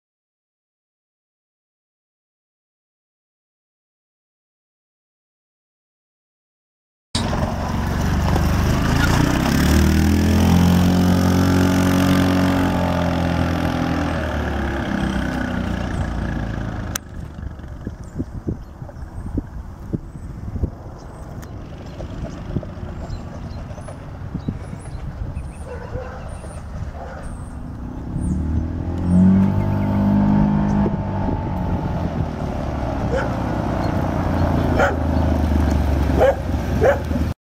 2012 Yamaha Rhino 700FI utility vehicle for sale | sold at auction April 22, 2015
2012 Yamaha Rhino 700FI utility vehicle
172 hours on meter
Yamaha 686cc four stroke gas engine
Liquid cooled
High, low, and reverse gears
Four wheel drive
On-Command
Digital console
Electric start
Molded entry doors
Dump bed
Locking tailgate
Front headlights
Seat belts
Roll cage
Front brush guard
Rear hitch bracket
Passenger grab handles
12V adapter outlet
Parking brake
7.9 gallon fuel tank
1,200 lbs towing capacity
15x10.00-12 off-road tires
1,199 lbs dry weight
5Y4AM16YXCA021424

It was located in Antlers, OK, and we sold it to the highest bidder for $6,930.